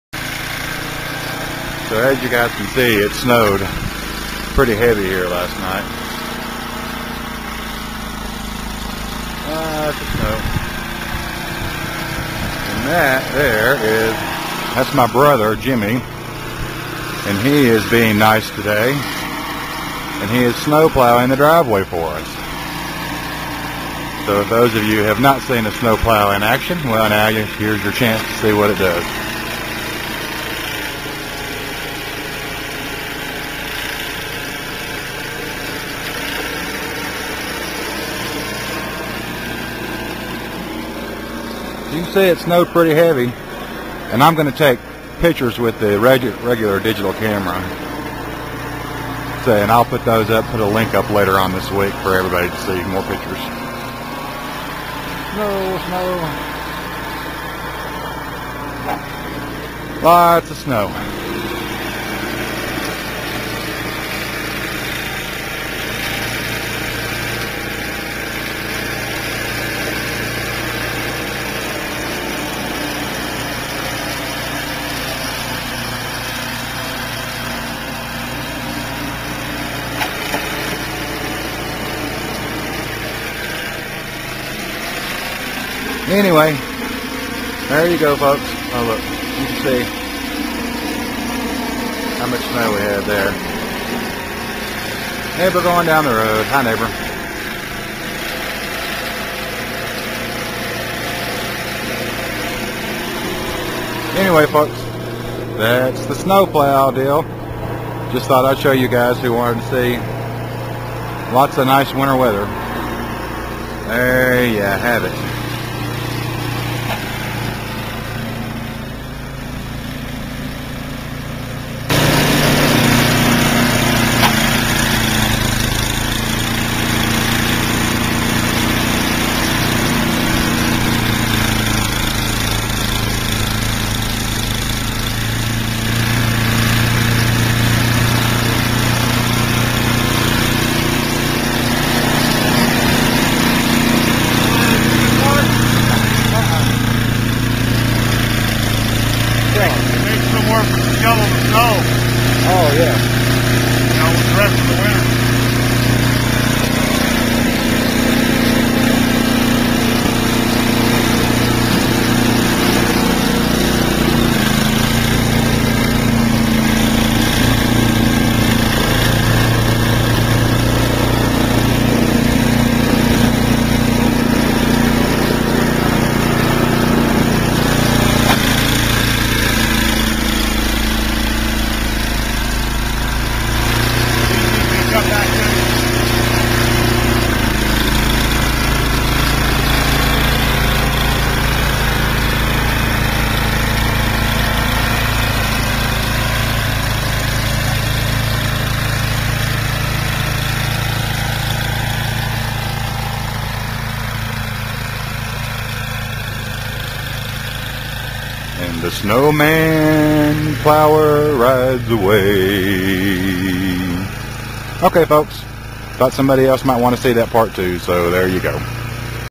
Brother Jim Sears Craftsman Snow Blower Demonstration - kalispell montana 2010
just my brother jimmy showing me how the sears craftsman snow blower works. He is clearing off the drive way and walkway after we had a huge snow storm here in the flathead valley. This is more of a demonstration than a review, but it sure looked like it worked pretty good to me.

Big fork, whitefish, swan lake, flat head, columbia falls, polson